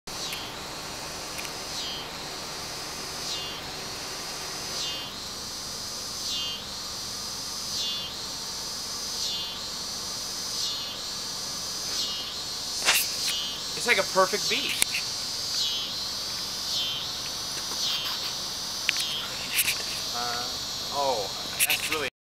Taken by Trees, or, the buzzing coming from treetops in Defiance, OH.
The search for the perfect beat. Help, I've been taken by trees.